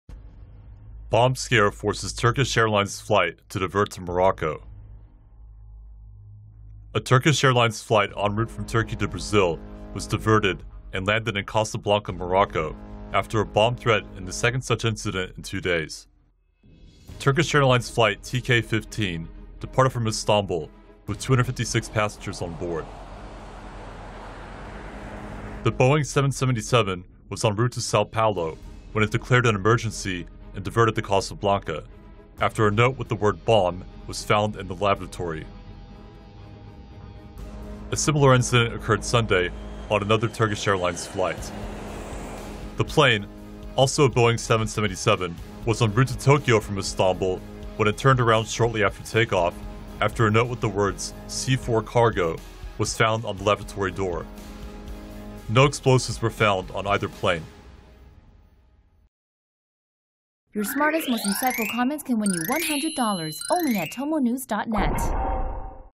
Bomb threat forces Turkish Airlines plane to divert from Sao Paulo to Casablanca
A Turkish Airlines flight en route to from Turkey to Brazil was diverted and landed in Casablanca, Morocco after a bomb threat in the second such incident in two days.

Flight TK15 had departed from Istanbul with 256 passengers and was flying to Sao Paulo when it declared an emergency and diverted to Casablanca after a note bearing the word "bomb" was found in a lavatory, a spokeswoman for the airline told Reuters.

The Boeing 777 landed in Casablanca at 12:30 p.m. local time. AFP reported that passengers were immediately evacuated upon landing, and a search for explosives was immediately launched.

No explosives were found and the plane was cleared to resume its flight.

On Sunday a Turkish Airlines flight from Istanbul to Tokyo returned to Istanbul shortly  after takeoff when a note that read "C4 Cargo" was found on the lavatory door. C4 is a type of plastic explosives.

No explosives were found on that plane either.